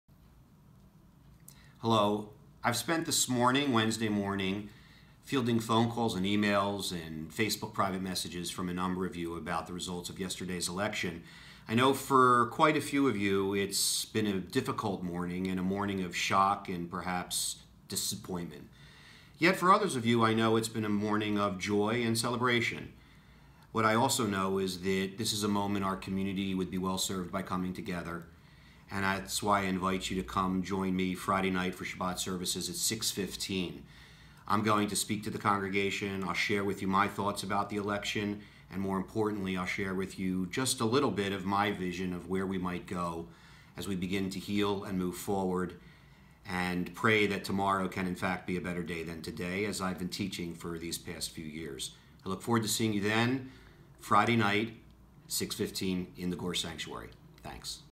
An Invitation from Rabbi Joshua M. Aaronson
Join Rabbi Aaronson Friday November 11, 2016 at 6:15 for Shabbat Services and thoughts post election.